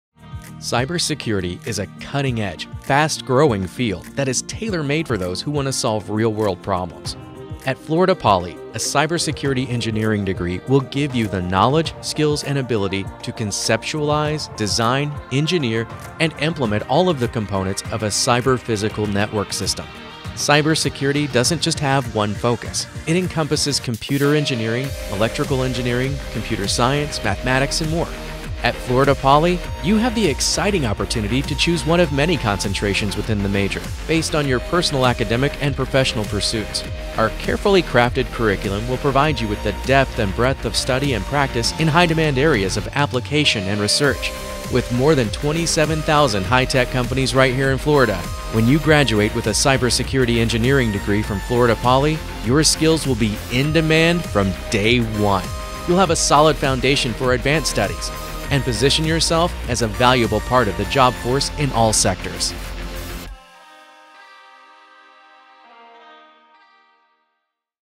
Cybersecurity Engineering: Solving Real-World Problems
Florida Poly's B.S. in Cybersecurity Engineering addresses security concerns for cyber-physical systems (CPS) and Internet of Things (IoT) devices.